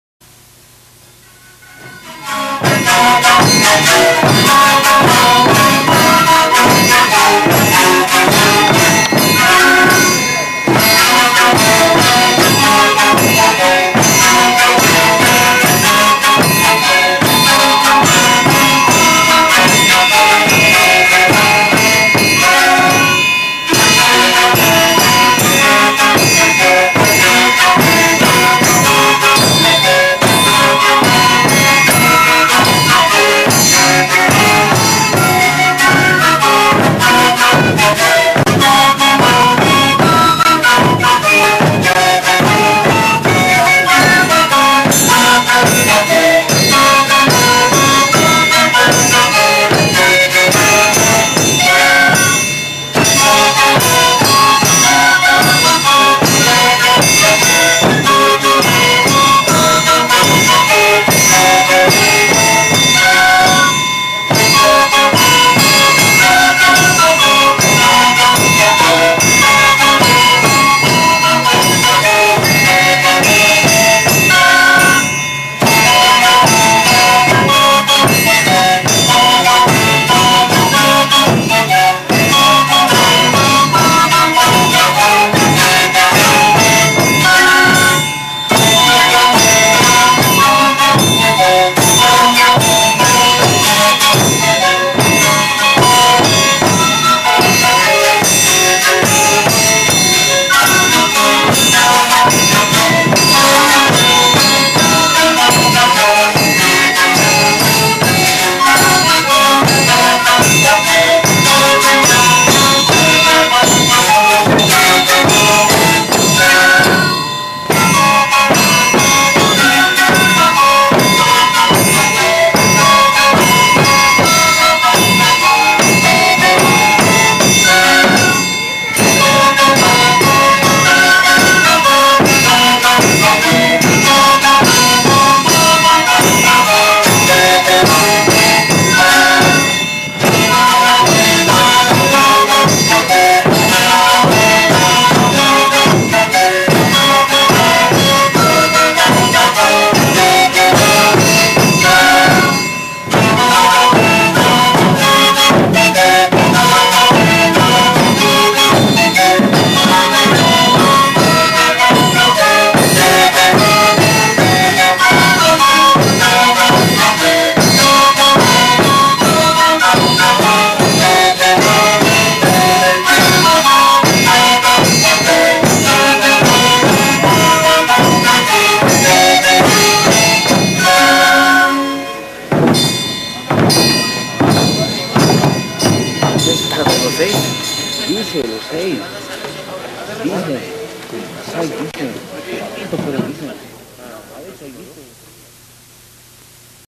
08 - Kantu de Niño Corin - Charazani 1998
Comunarios de Niño Corin - Charazani
El Presente Video es parte de una serie de Filmaciones que realizara Alejandro de Luca, fundador de Wayramanta, en la fiesta de la Virgen del Carmen en Charazani, Departamento de La Paz, a fines de los años 90.
 Originalmente fue grabado en VHS y luego digitalizado por nosotros.